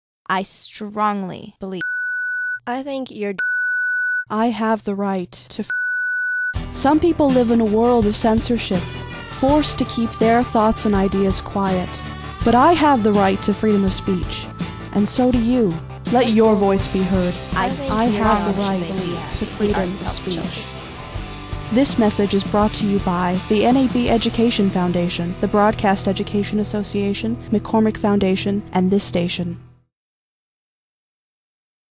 NAB Freedom of Speech PSA
My entry for NAB's annual Freedom of Speech PSA contest. (Which is also my PSA project for Intro to Media Production.)

CREDITS
Written / Edited / Narrated by...Laura Abbott
Substantial Editing Help (and help in general)...Prof. Jeff Bradbury
Voice #1 ("I strongly believe...")...Kelsey Burston
Voice #2 ("I think...")...Sabina Cao